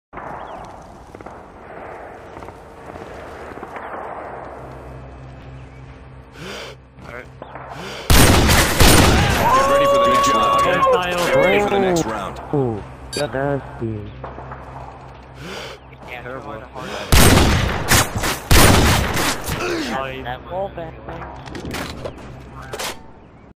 Play of the Week #3
Just a nice little wall bang in S&D. Hope you enjoy